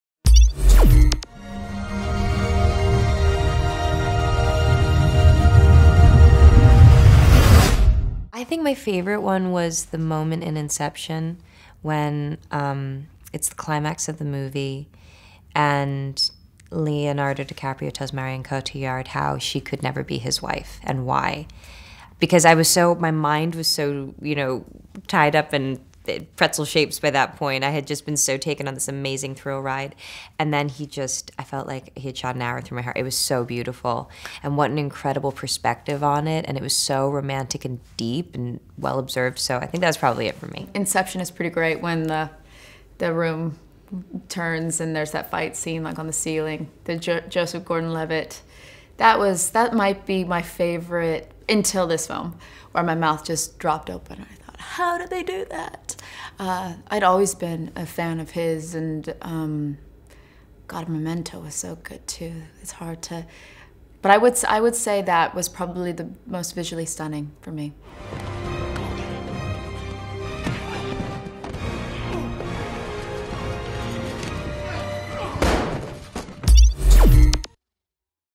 Anne Hathaway and Jessica Chastain's Favourite Christopher Nolan Movie Moments
Stars of Interstellar - and fans of Inception - Anne Hathaway and Jessica Chastain pick their favourite moments in Christopher Nolan movies.